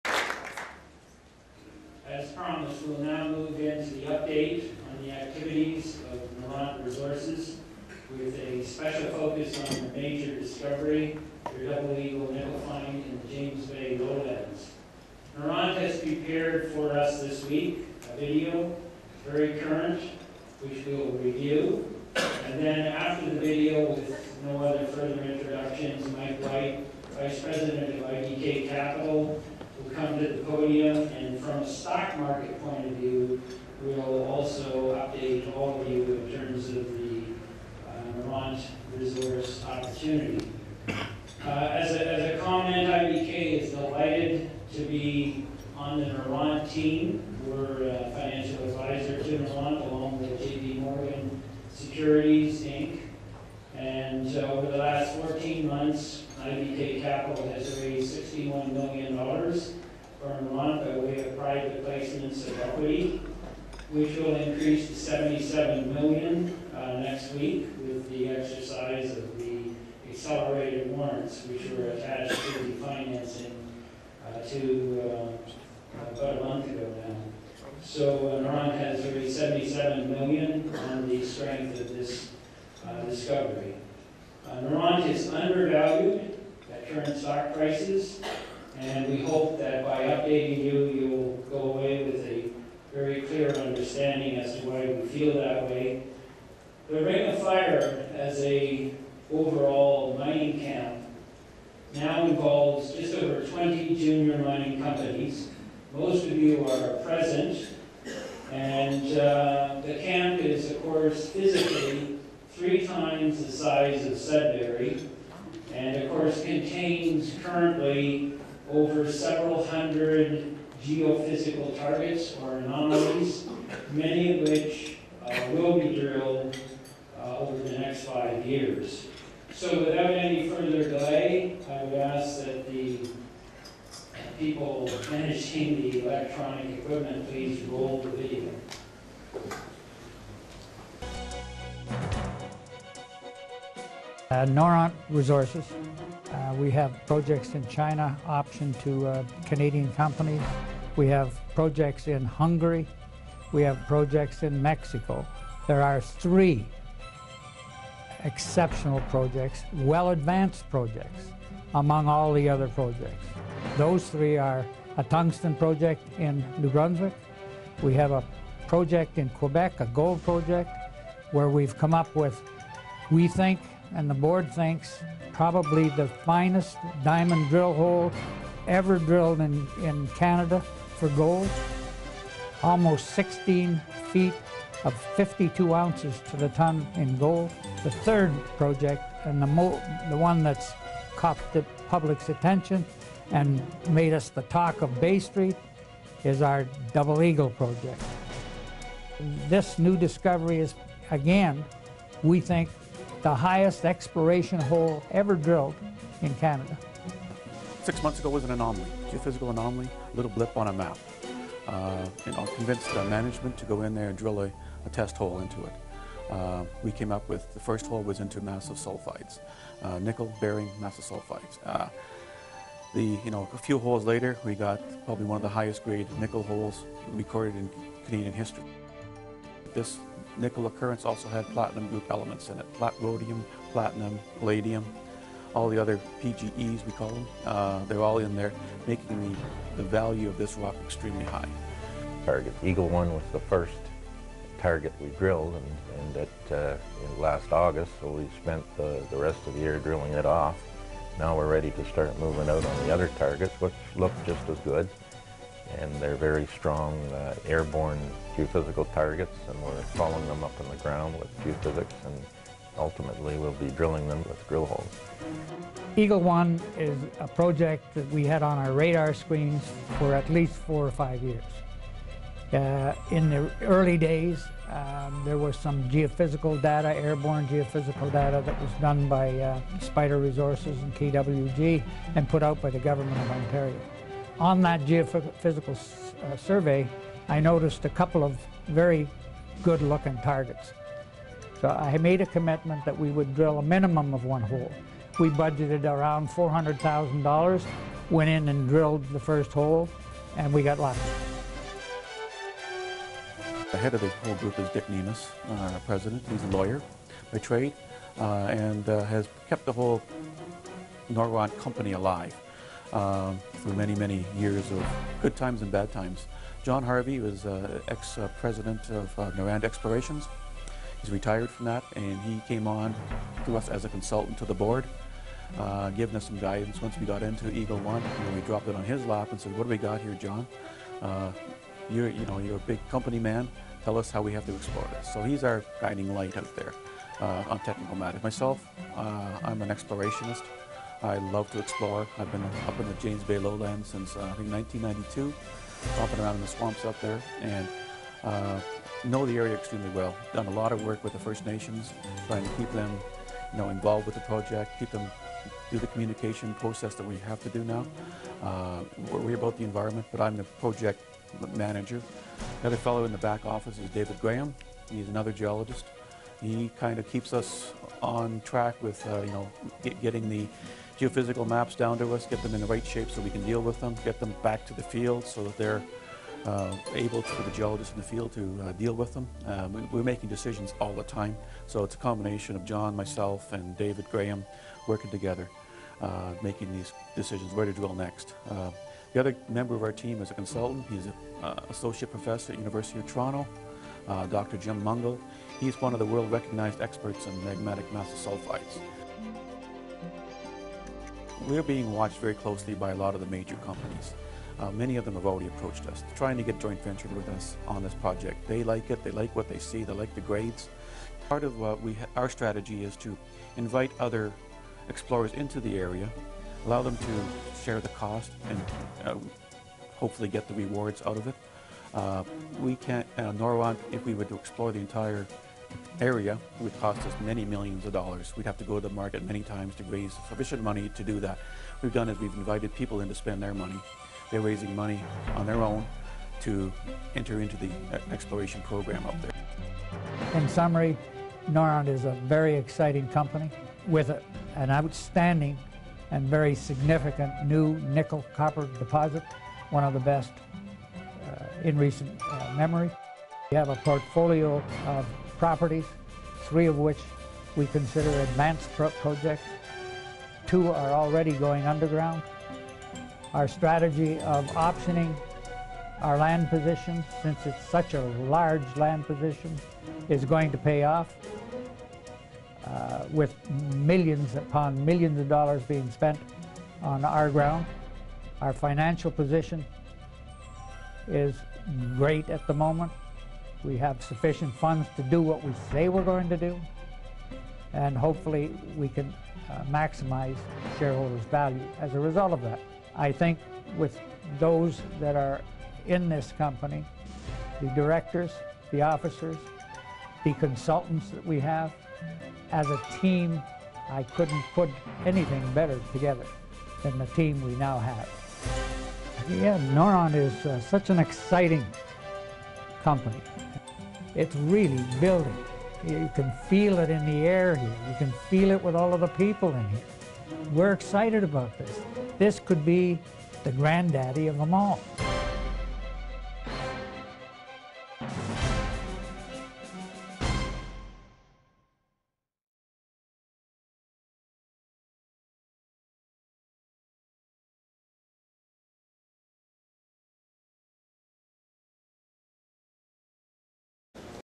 IBK Presentation - Part 1
IBK presentation at the Ring of Fire Luncheon at the Royal York Hotel.